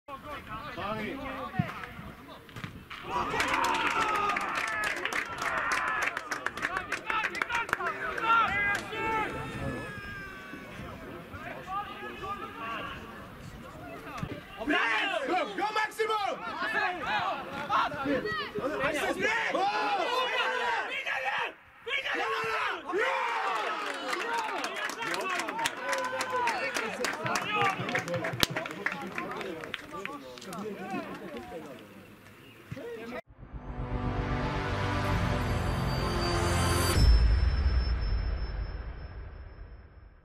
Ararat-2 - Alashkert-2 2:0
Armenia First League
Goals: Hayk Sargsyan 51 (own goal),  Harutyun Harutyunyan 74